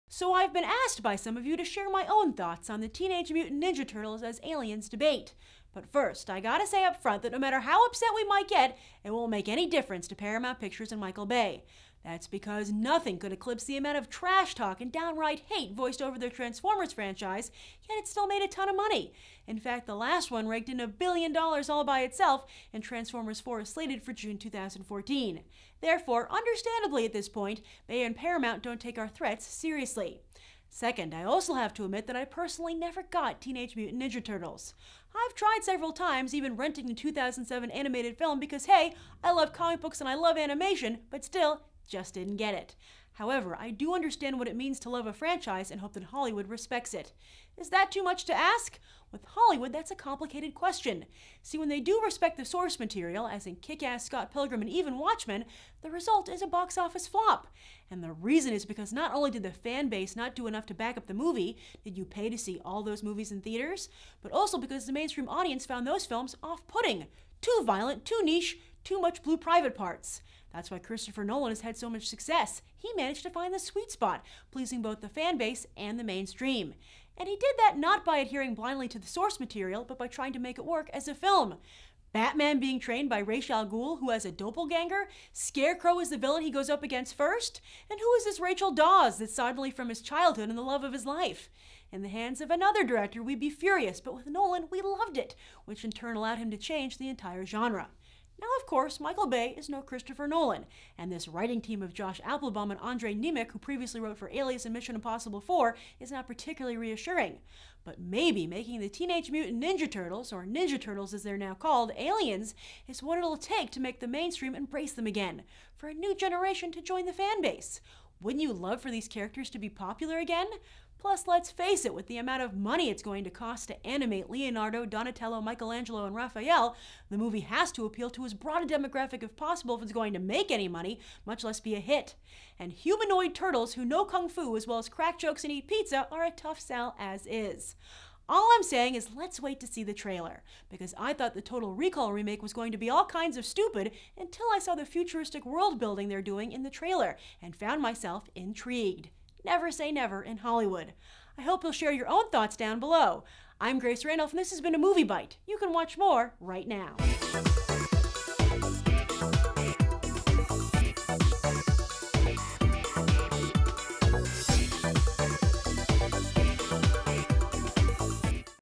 Ninja Turtles Movie 2013 : Teenage Mutant turned Aliens! - Beyond The Trailer
Teenage Mutant Ninja Turtles are returning to the big screen in 2013 courtesy of Michael Bay and Paramount Pictures.  But instead of being Teenage Mutants they'll now be aliens!  Beyond The Trailer host Grace Randolph gives her take on this controversial decision that has fans up in arms!  But maybe aliens are a good idea?  What's it going to take to make Teenage Mutant Ninja Turtles popular again, especially in 2013?  Beyond The Trailer is associated with YouTube Next Lab.